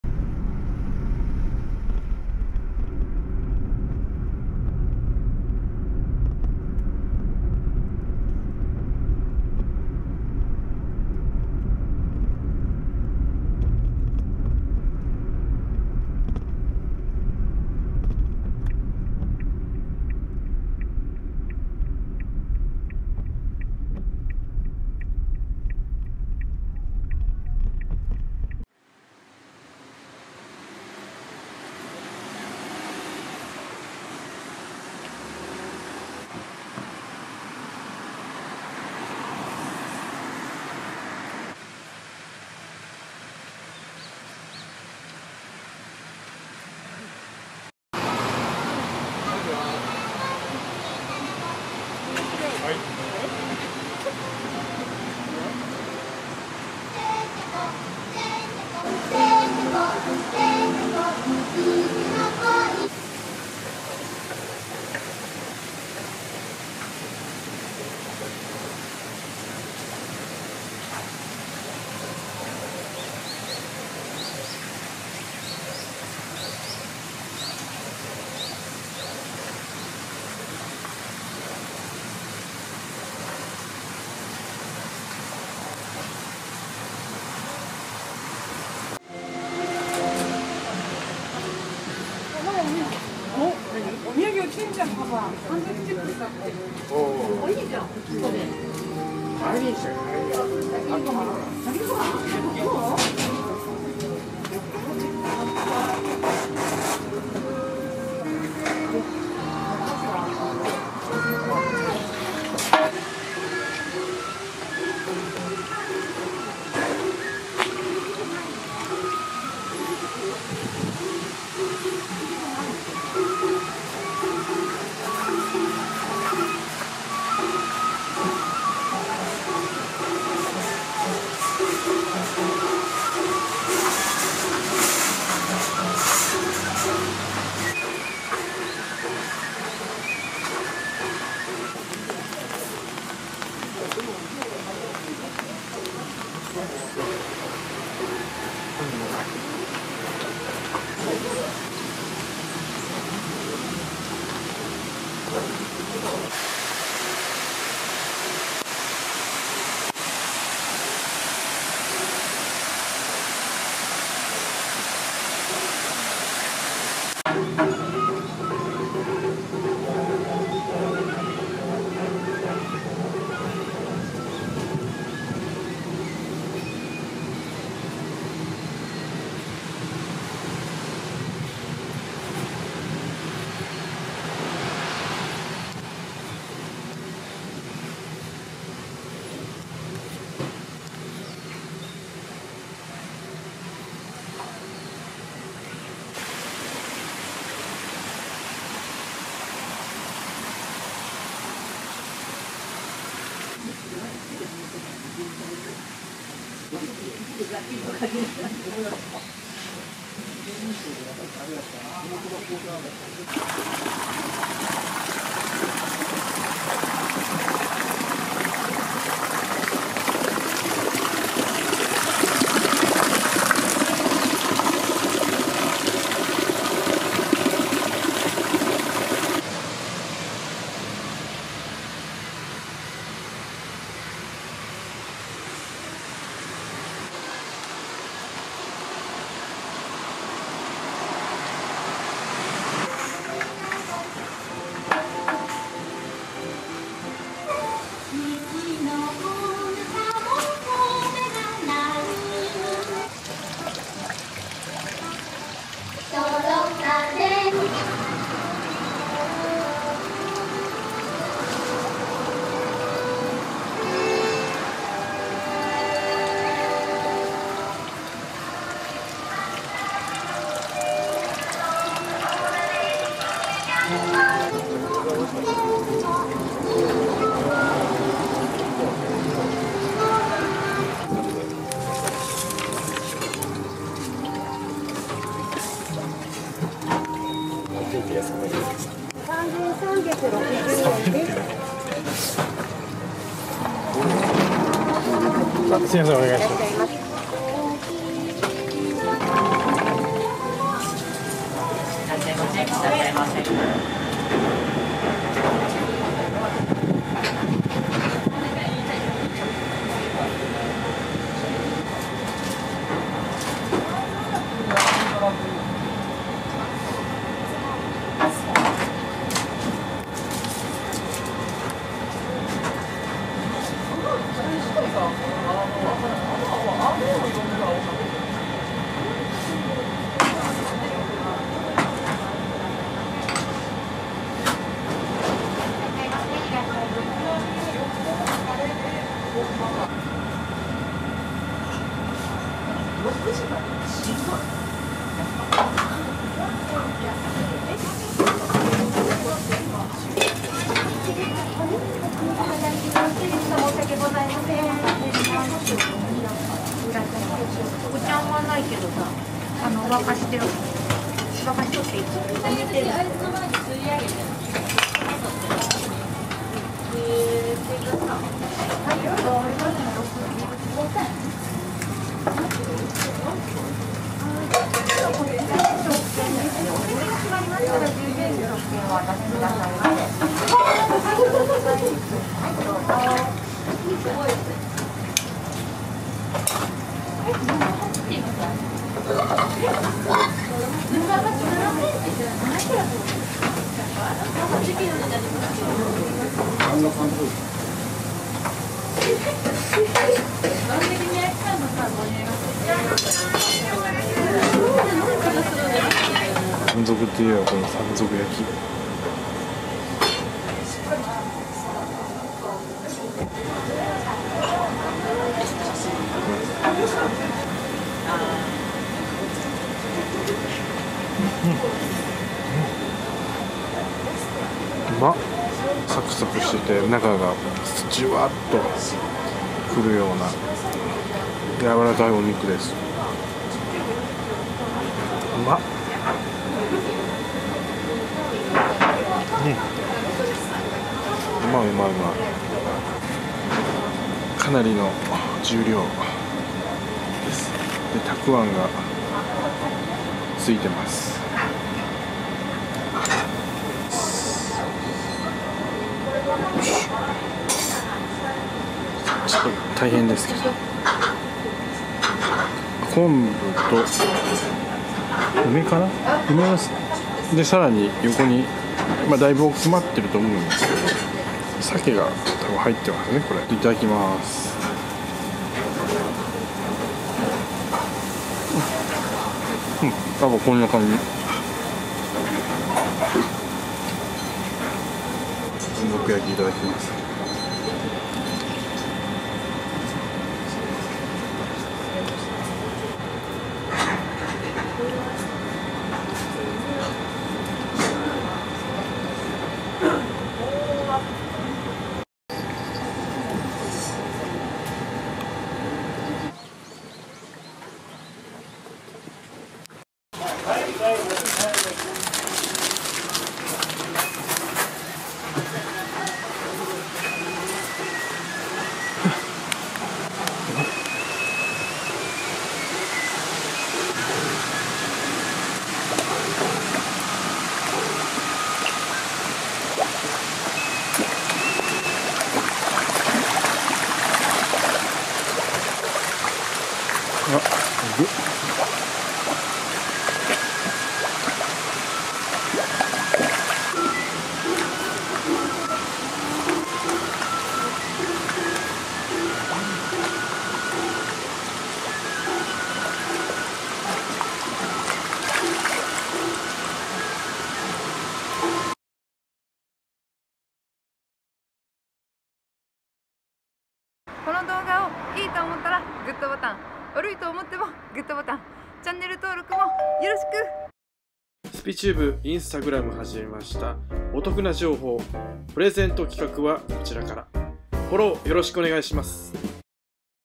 山口【いろり山賊 玖珂店 】ドライブインチックな感じで存在！山賊焼&山賊むすびを注文！一度は絶対に行くべし！
山口県岩国市玖珂町1380-1

再生リスト